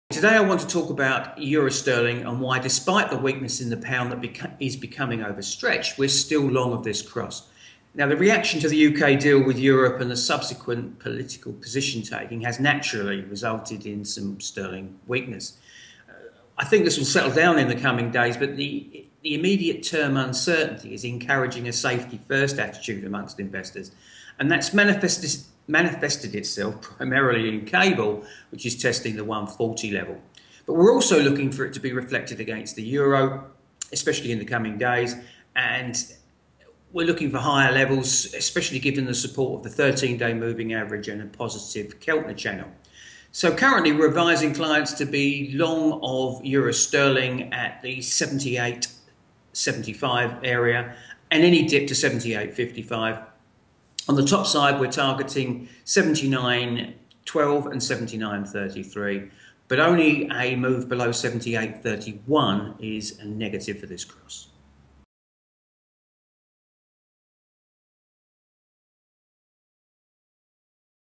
Collins: Why I am long EURGBP
Alan Collins is buying EURGBP as he believes it is becoming overstretched despite weakness in the pound.

All the uncertainty around the UK’s future as a member of the EU has resulted in selling of the pound, and while things are likely to settle in the coming days, Collins says the current uncertainty is encouraging a safety first attitude among investors.

This is most visible in GBPUSD, but is also likely to be reflected in the euro, Collins says. He is looking to go long at the .7875 area and on any dip to .7855. His targets for the trade are .7912, .7933 and .7955 with a stop below .7831.